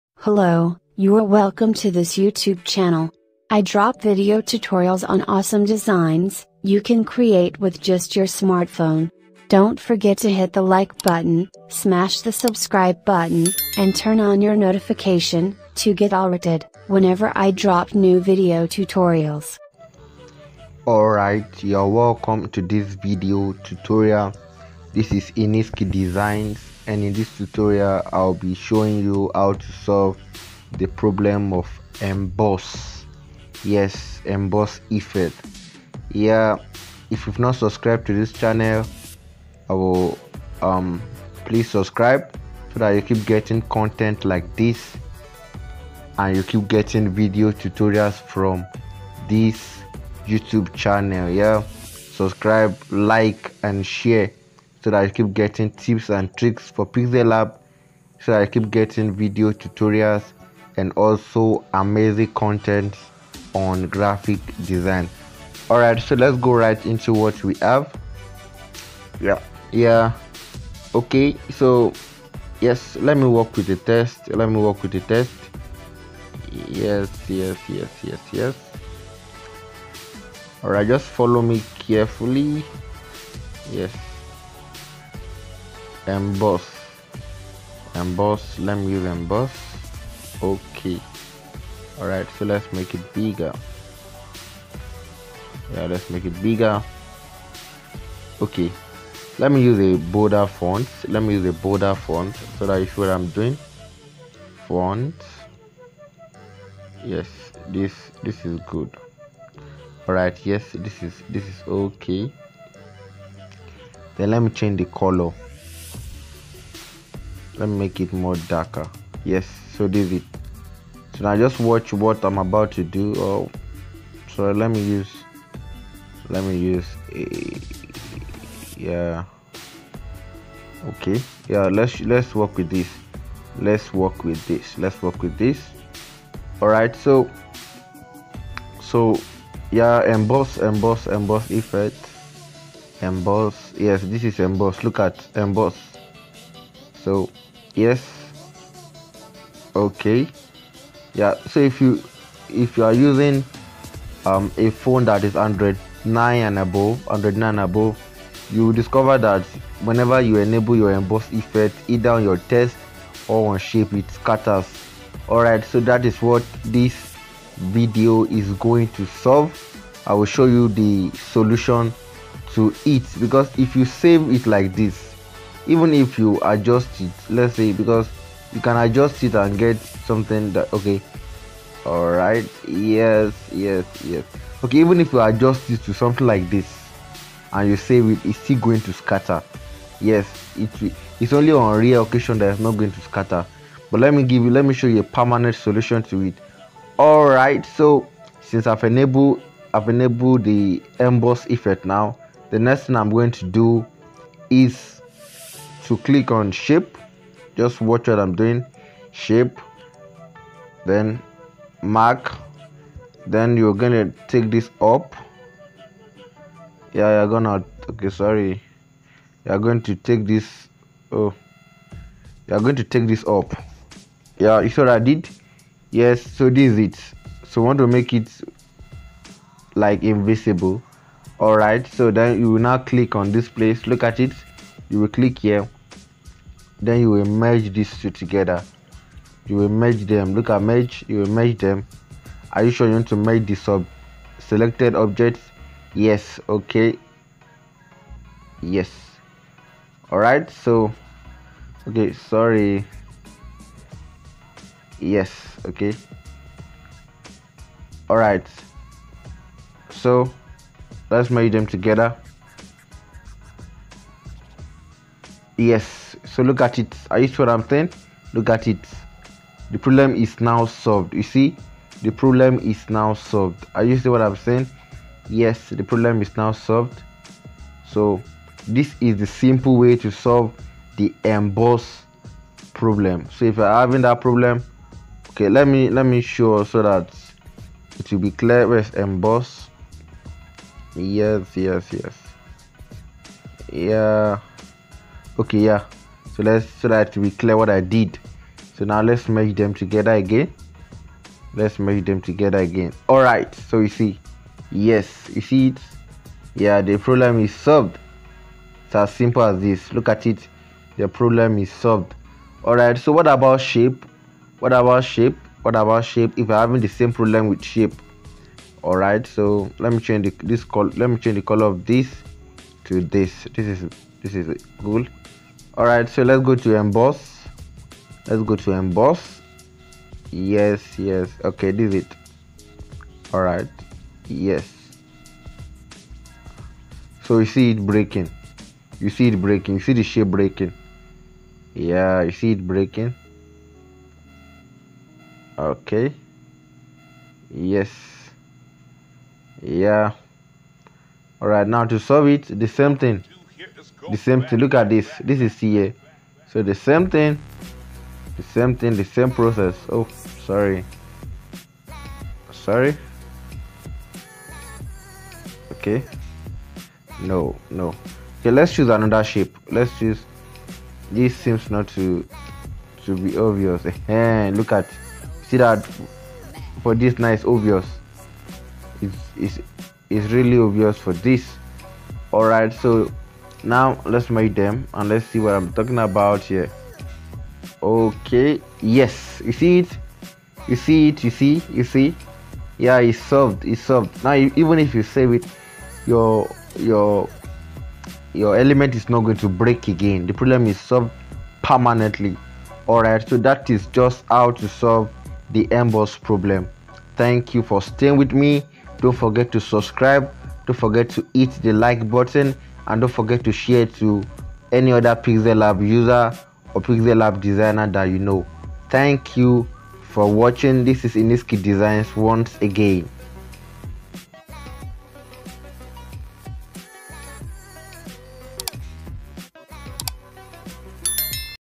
EMBOSS PROBLEM ON PIXELLAB SOLUTION | PIXELLAB TUTORIAL
A lot of pixellab users especially those using smartphones with android 9 version and above struggle with EMBOSS because it keeps scattering the elements whenever they enable it. Some pixellab users has even stopped using it

Here is the solution, check out the solution in this video